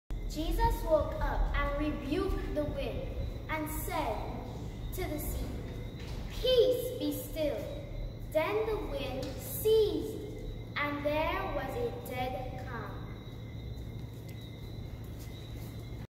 Christmas 2021 (1)
Under restricted public gathering due to Covid-19 Pandemic; the Sunday School sort creative ways to celebrate Christmas.   This collection is 2021 edition.